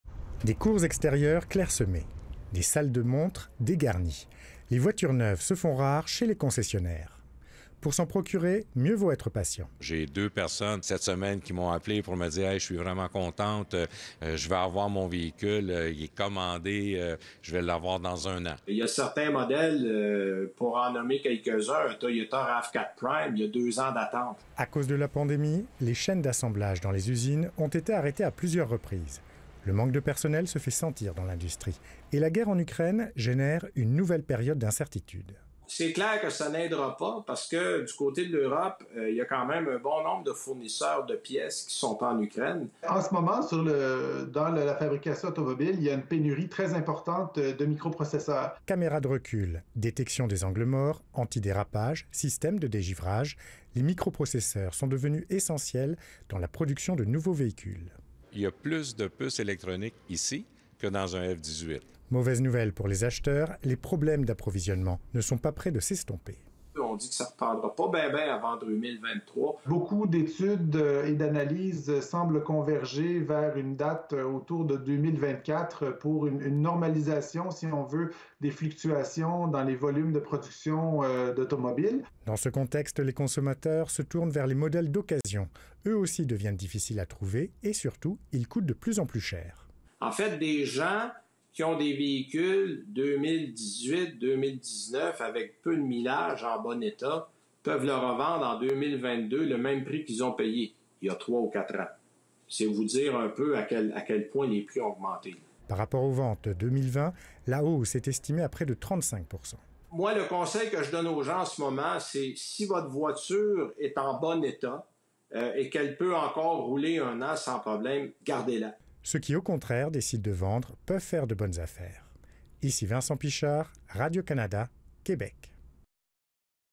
Les voitures à vendre se font rares
Acheter une voiture neuve ou d'occasion est devenu compliqué. Peu de modèles sont disponibles. Les prix sont élevés. Et tout ça n’est pas près de changer. La faute à la pandémie, alors que la guerre en Ukraine plonge l'industrie automobile dans une nouvelle période d'incertitude.

Suivez ICI Québec sur différentes plateformes